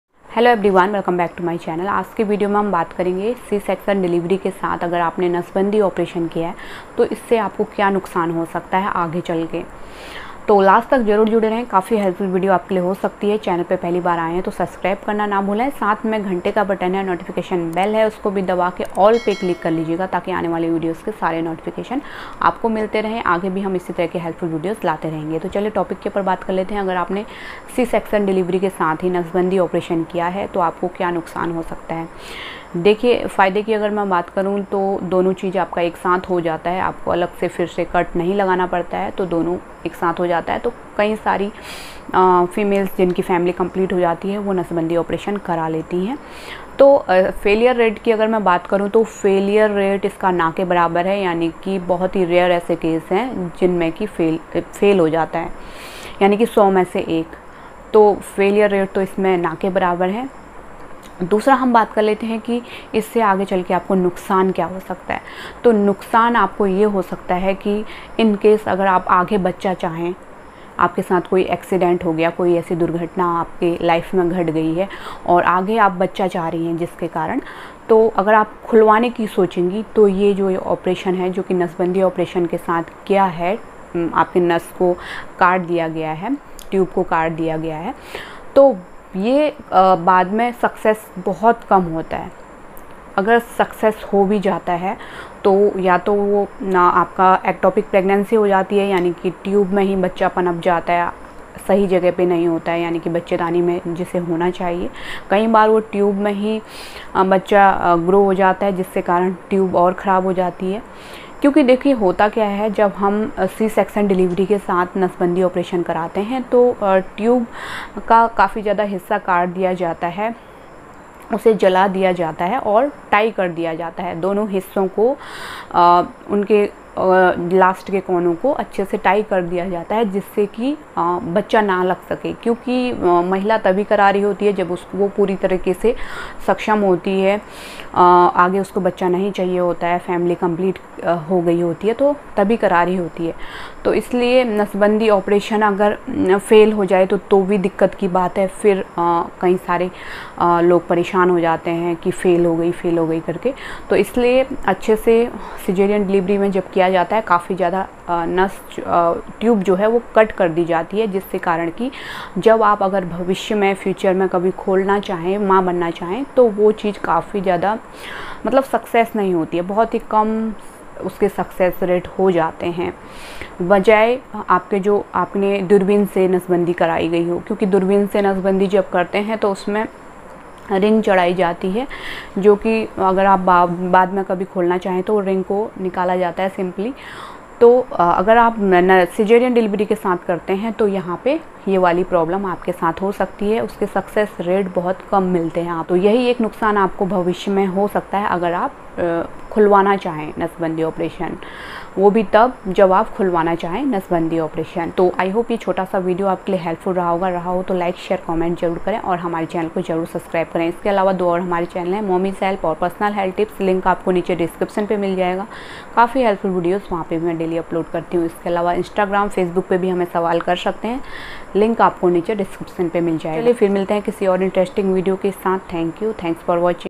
सिजेरियन डिलीवरी के साथ नसबंदी करवाने का नुकसान|Tubal Ligation With C- Section Delivery:Disadvantage
Today Topic: सिजेरियन डिलीवरी के साथ नसबंदी करवाने का नुकसान|Tubal Ligation With C- Section Delivery:Disadvantage

my Another Channel Link's:

"Mommies Help"